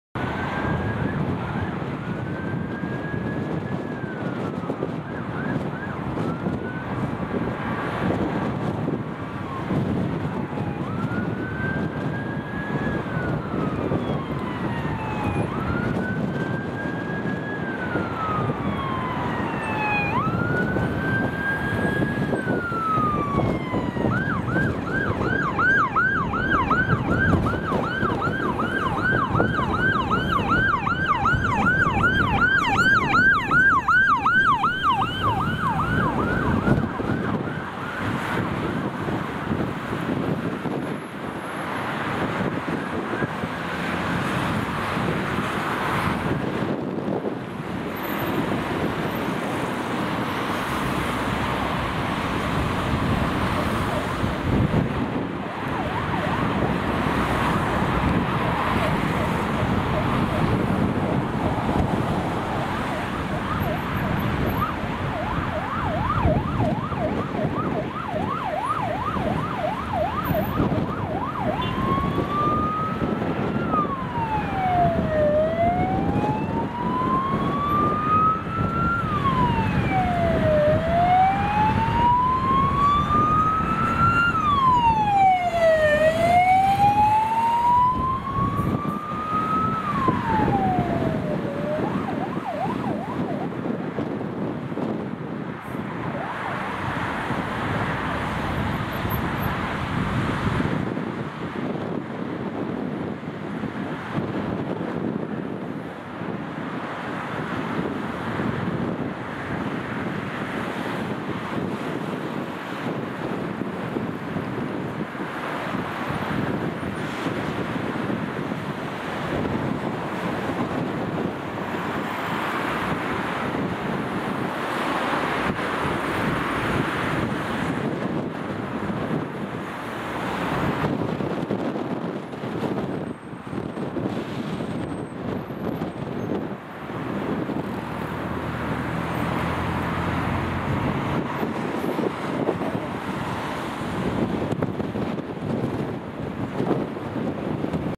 hovedstadens beredskab RTW A76 + falck ASF 6637 ambulance & fejeblad i udrykning
ambulance a76 fra hovedstadens beredskab + fejeblad 6637 fra falck i københavn

ambulance a76 from hovedstadens beredskab + tow truck 6637 from falck in copenhagen